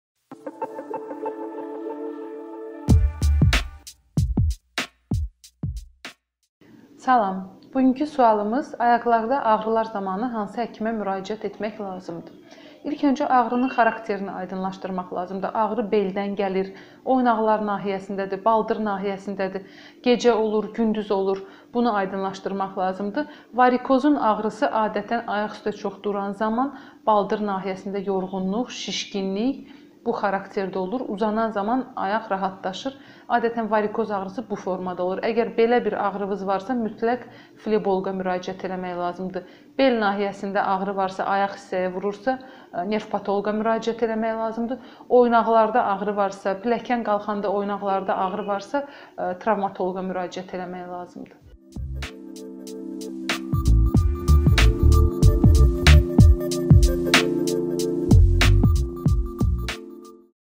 Ayaqlar ağrıyır, kimə muraciət etmək lazımdır? - Dr. Zəkiyyə Raufqızı
📍   Ak. Həsən Əliyev küç. 15, "Avrasiya Hospital", 2-ci mərtəbə